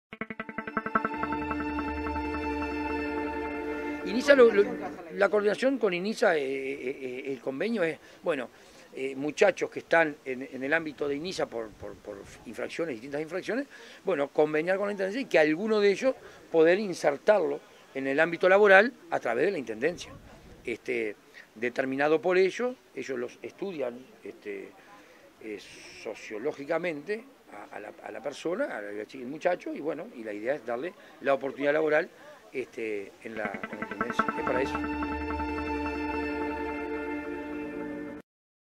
Convenio IMS - INISA
Jovenes amparados por el INISA podrán acceder a una  oportuni8dad laboral en la Intendencia de Soriano.
Tras una evaluación que realizará INISA del perfil social de cada uno de ellos.